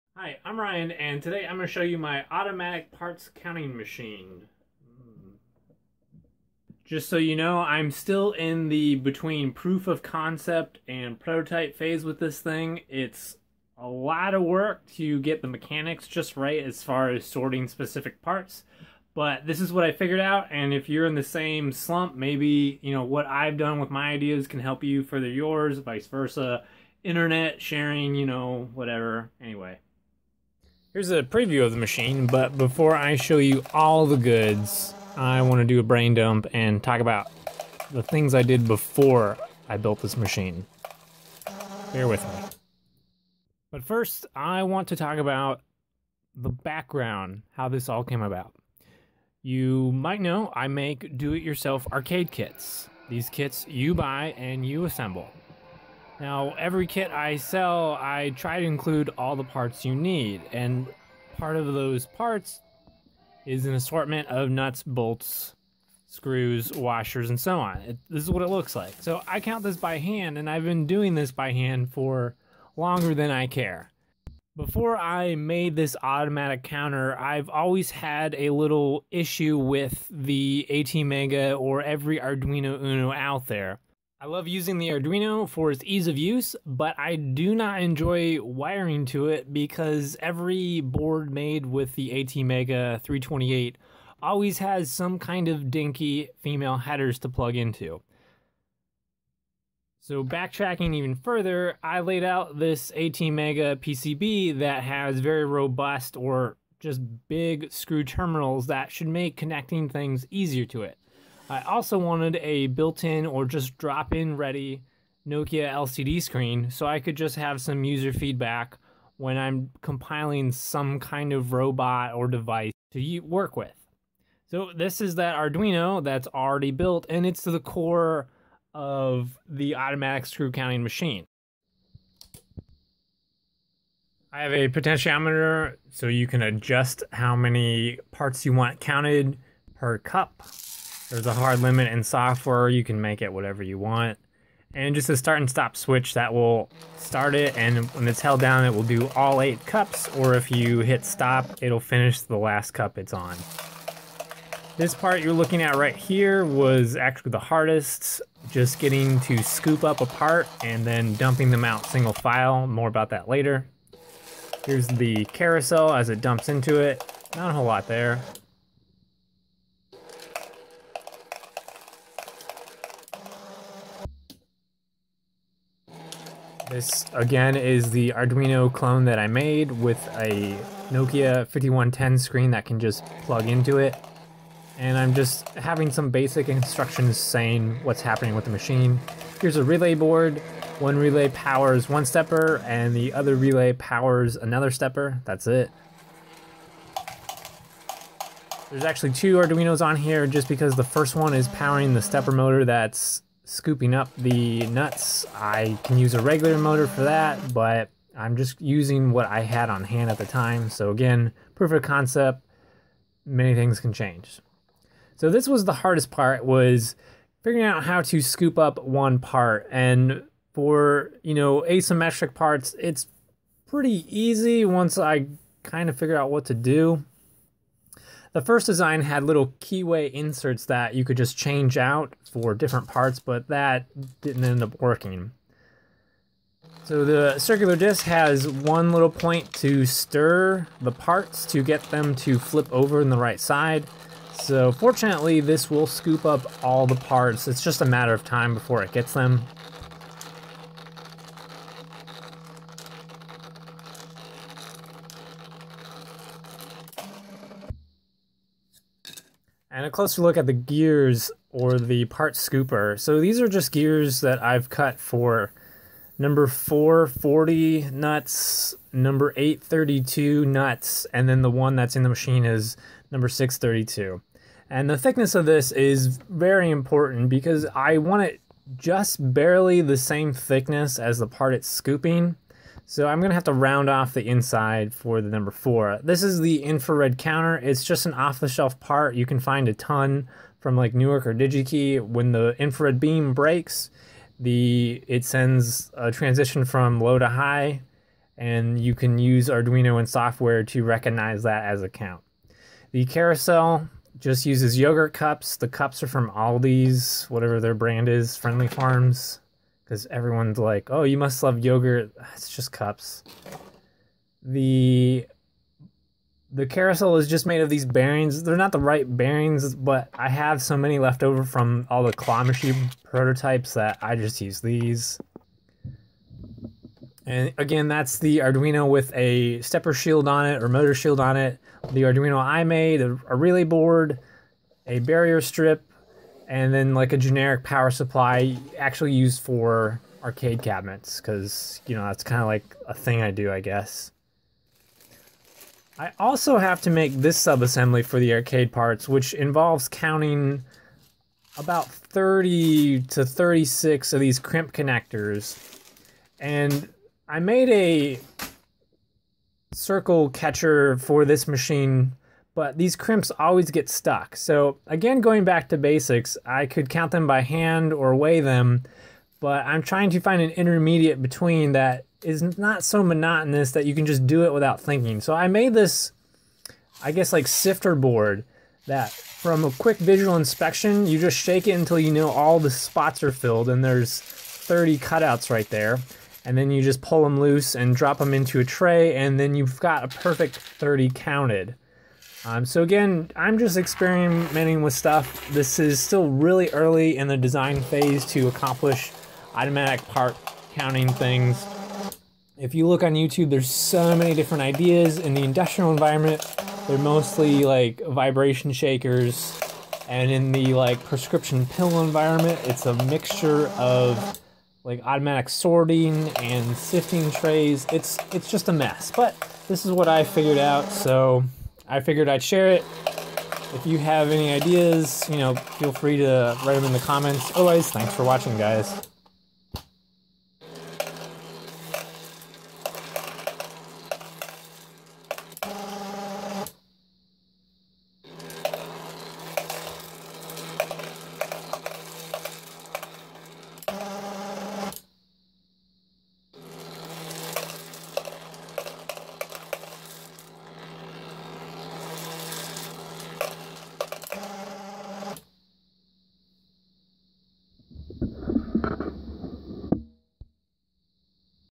Automated Small Parts Counting Machine with Arduino
A machine that counts and sorts set quantities of parts into individual cups- to replace a human [me] doing this repetitive task. Machine is automated. Once turned on it will count the predetermined set of small nuts (#4-40, #6-32, and #8-32) until completion. This machine is very early in design and inherently also very time consuming to develop. Just sharing my ideas to help other DIY-ers that may have the same redundant task of counting parts.
Weighing parts (the crimp connectors) is always an option, but I am trying to automate the task as an end goal, so counting seemed like the correct first step. It's a learning process.